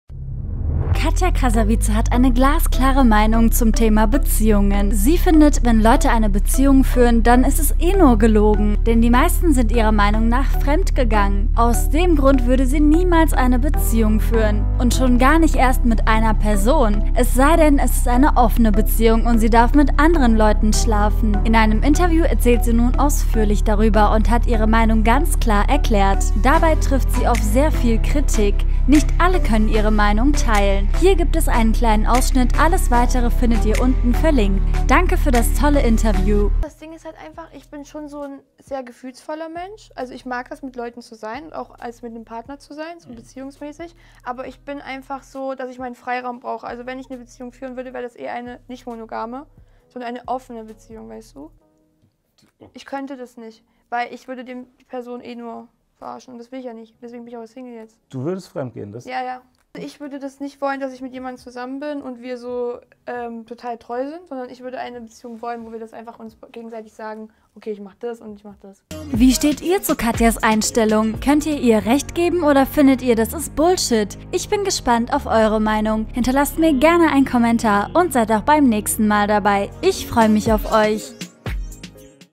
KATJA KRASAVICE will mehrere Typen in einer Beziehung haben 💔 Sie kann nicht treu bleiben!
*Song kostenlos Streamen*
Hier klicken

Infos zum Video:
KATJA KRASAVICE hält nichts von einer Beziehung, selbst wenn sie in einer sich befindet, würde sie Fremdgehen, wie sie nun bekannt gibt.